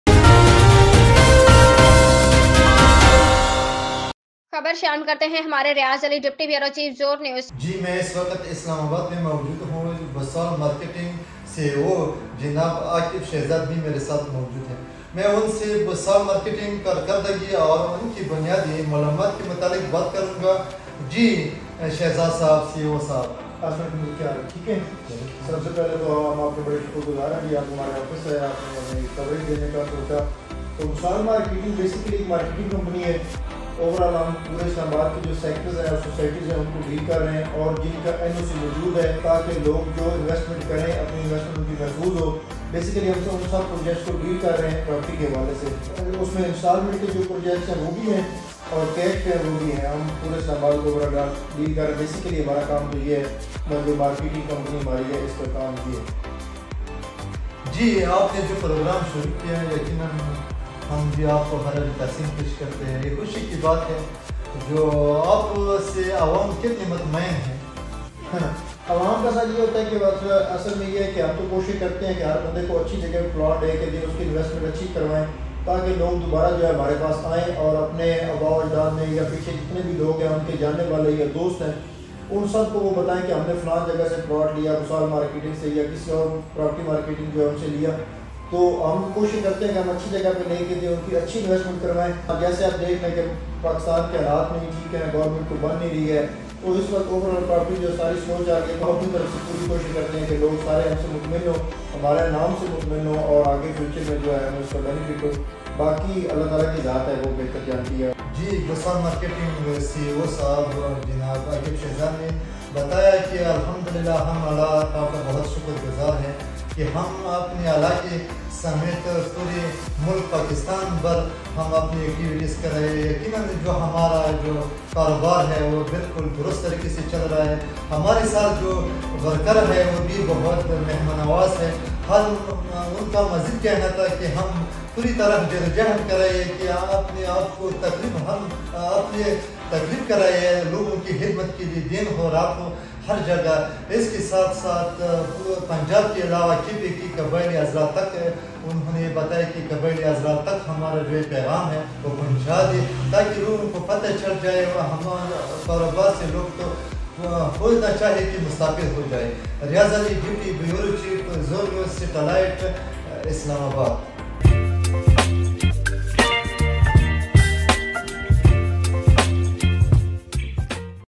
Deputy Bureau Chief (Riyaz Ali) Asking Some Question To Akif Bosaal (Ceo) Related To Realestate.
Deputy Bureau Chief (Riyaz Ali) Zoor News Asking Some Question To Akif Bosaal (Ceo) Related To Realestate

Had A Nice Meeting With Deputy Bureau Chief (Riyaz Ali) Zoor News.Thanks For Giving Us Time And Thanks For Wonderful Interview.

For Details & Booking:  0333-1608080 / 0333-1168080